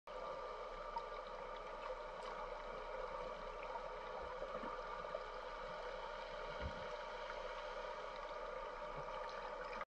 Otter swimming by
Filmed using a GoPro Hero 9 underwater.  Lowered from the surface and left in place for 10 minutes.  No guiding and no monitoring.  Filmed at the Port Townsend Marine Science Center.